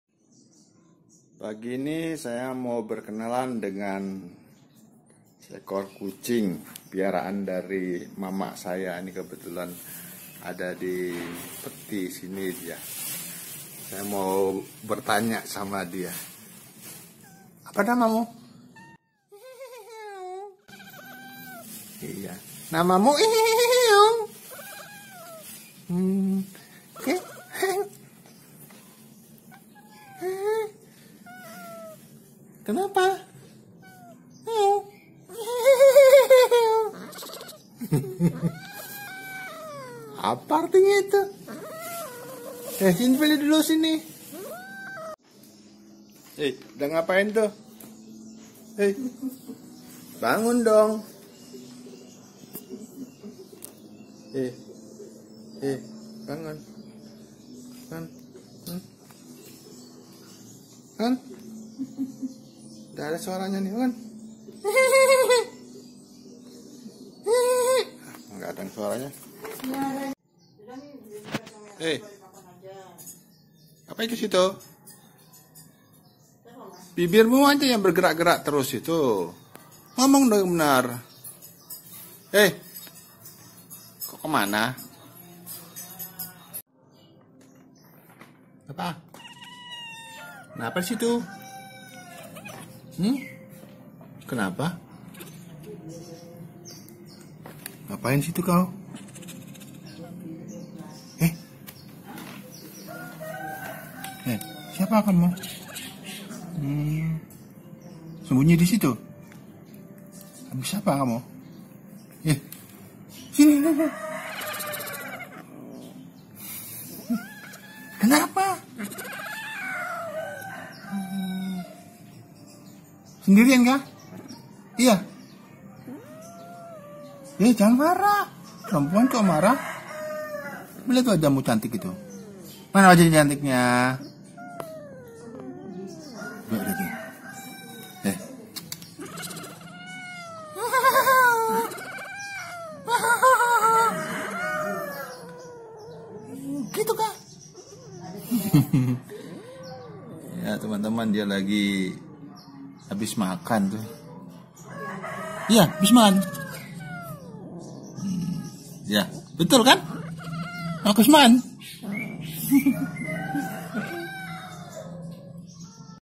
Berteman sama kucing baru
Berteman sama kucing baru. Ketika saya sedang mengunjungi ke rumah orangtua yang sudah sangat tua. Ternyata diteman se-ekor kucing ywng selalu menjawab pertanyaan aku. Mari kita saksikan videonya.